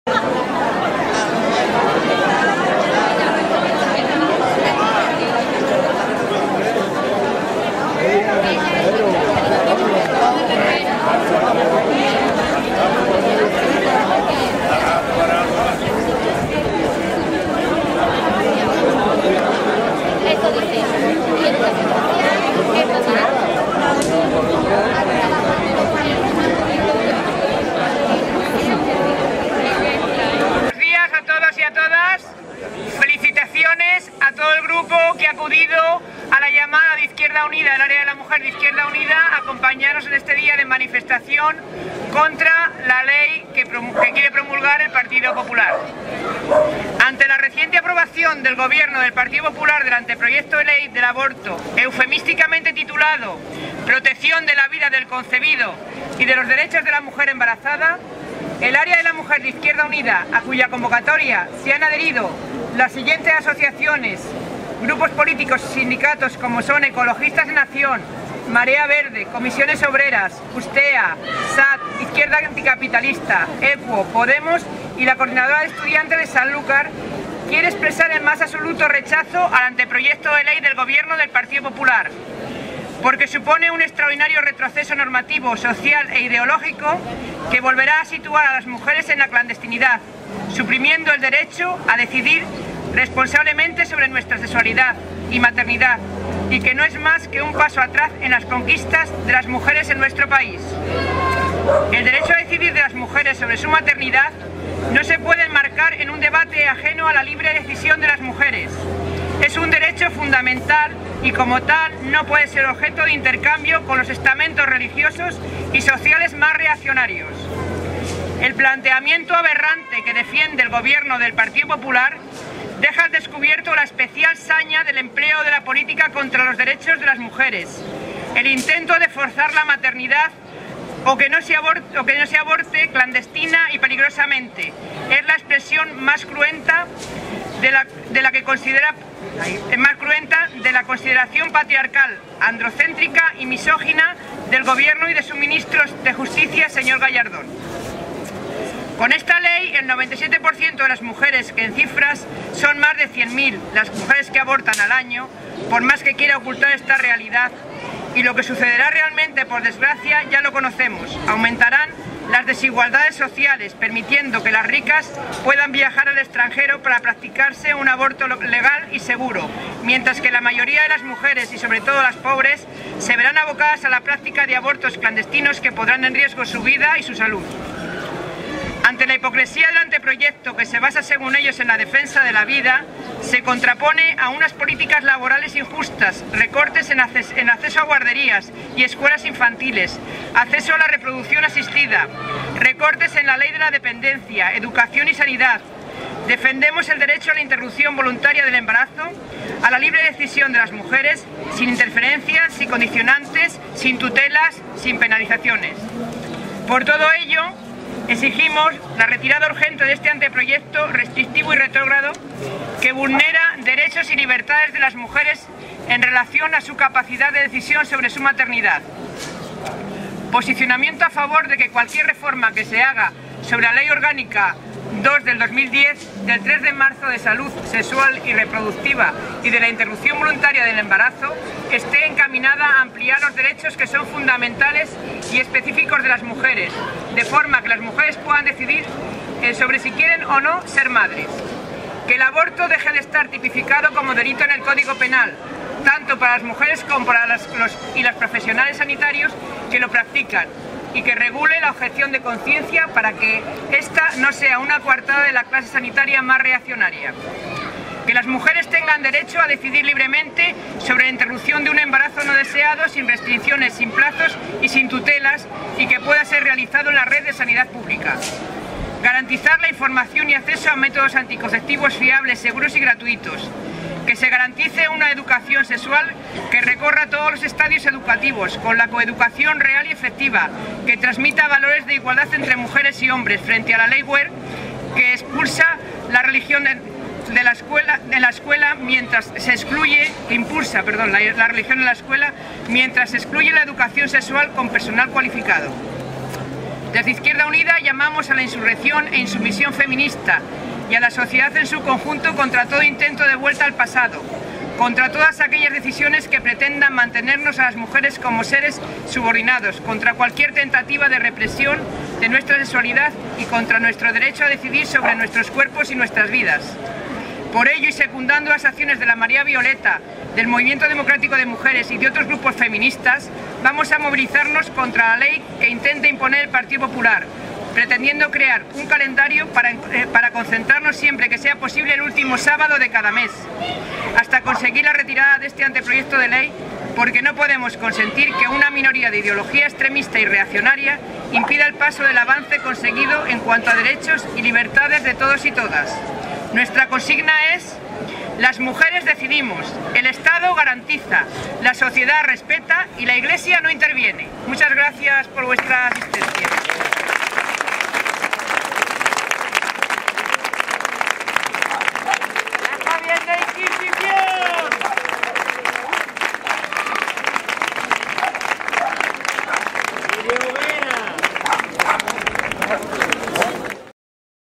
Concentración contra la reforma de la Ley del aborto
Unas 150 personas se dan cita en los alrededores de la Biblioteca municipal de Sanlúcar, convocadas por Izquierda Unida para protestar por la reforma dde la Ley del aborto